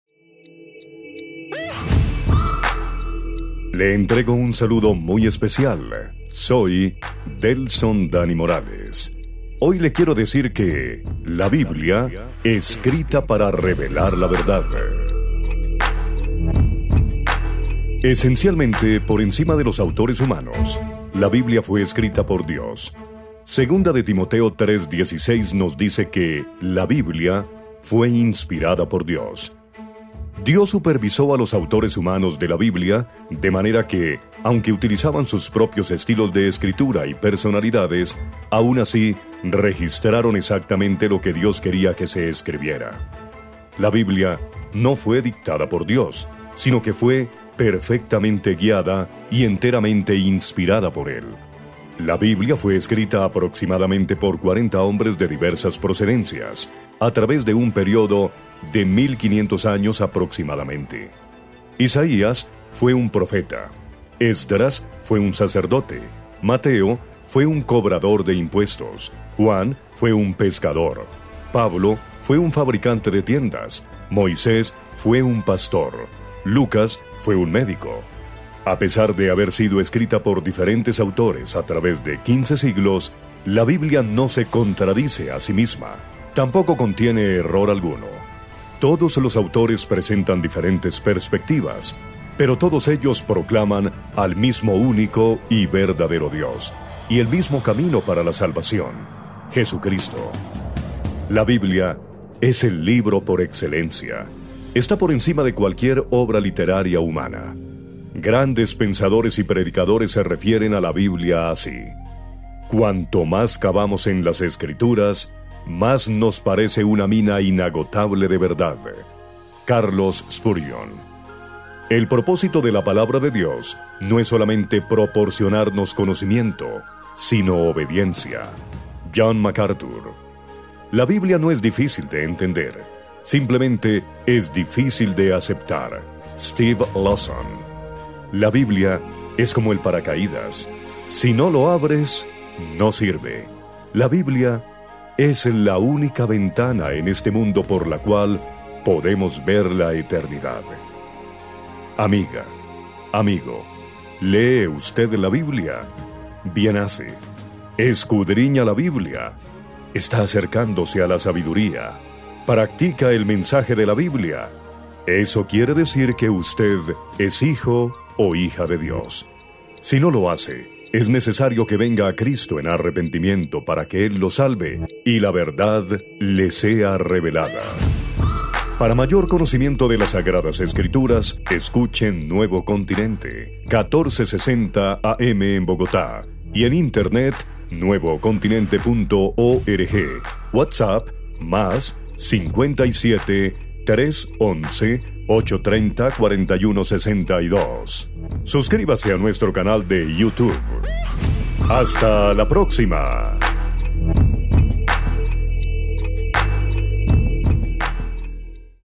La Biblia: Escrita para revelar la verdad - Reflexión | Emisora Nuevo Continente
Reflexión con Delson Morales para este miércoles 13 de octubre de 2021